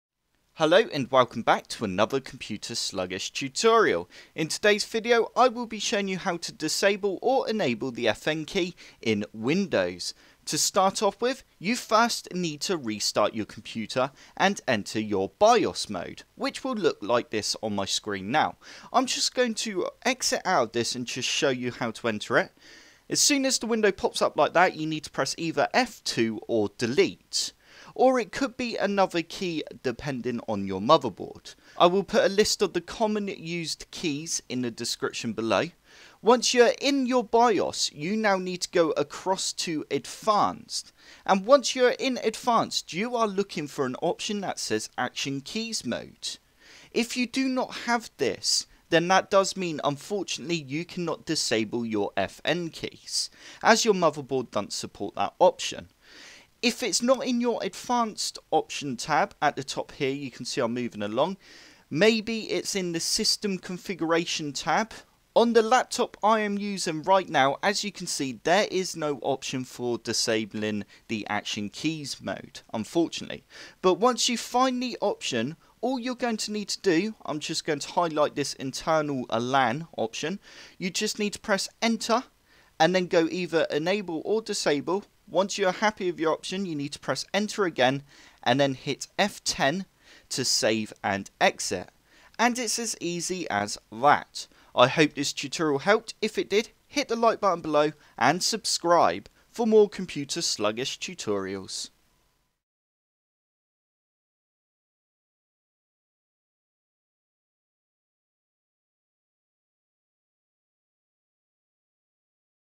How To Enable & Disable FN Key On Laptop | Enable or Disable Function Keys
In this Tutorial I will be showing you how to disable the FN key on laptops you will need to do this in the bios but not all bios have the setting to do this like mine in this video.

Common keys to enter BIOS:
F1
F2
Del
Esc